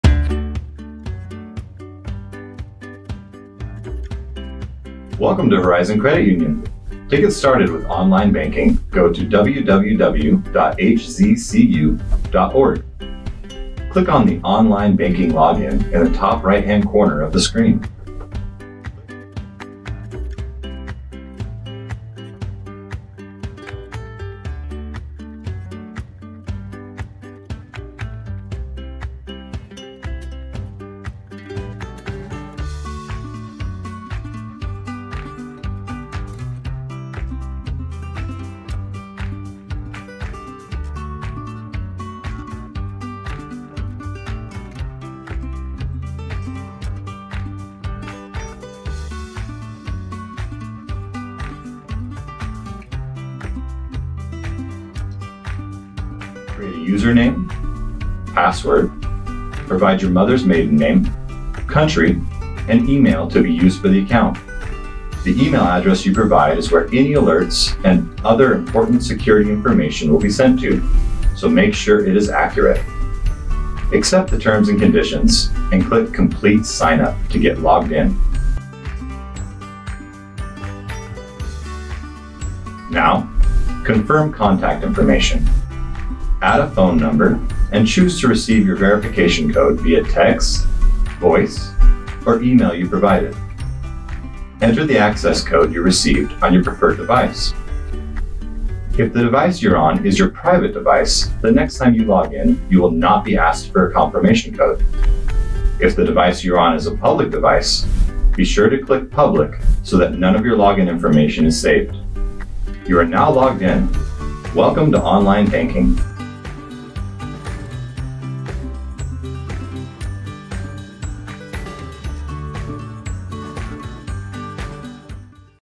Getting Started with Online Banking
Guided tutorial for getting started in Online Banking with Horizon Credit Union.

This is the place to start for all things digital at Horizon. See the next video details on getting set up for the first time. From there, all of our digital tools are at your fingertips!